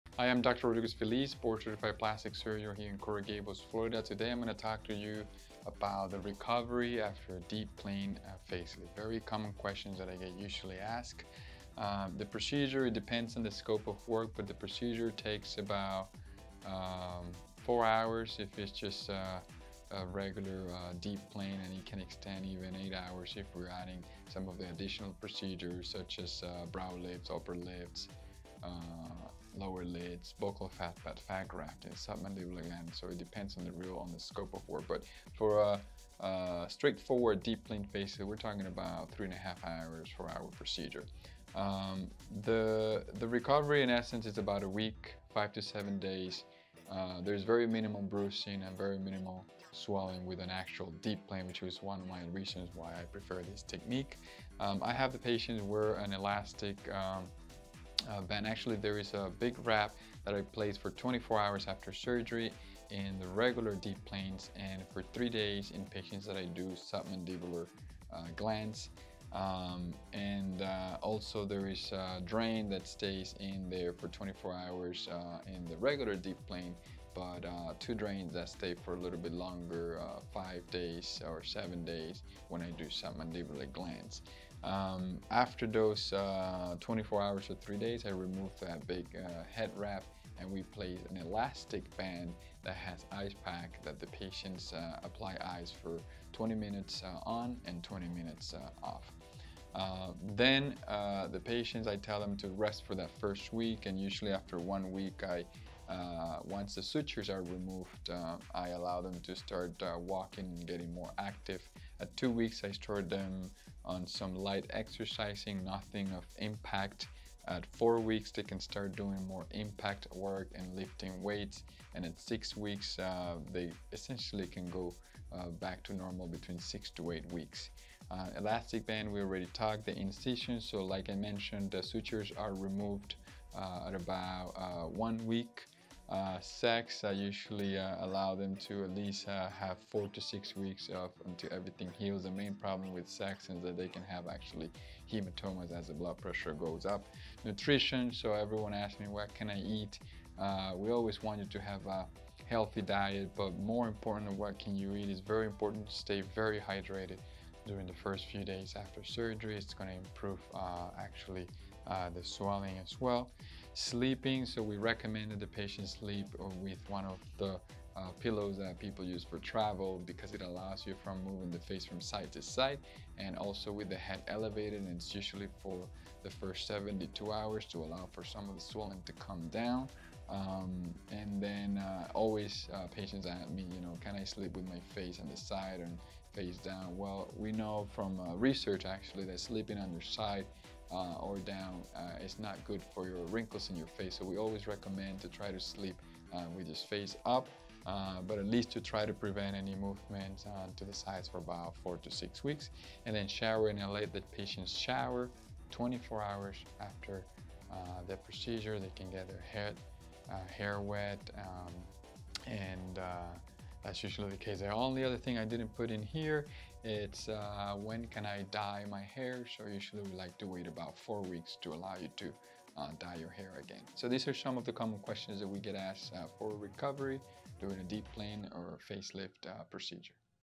How long do I have to wait to be intimate after a facelift?
This and other questions were answered about the recovery after a deep plane facelift.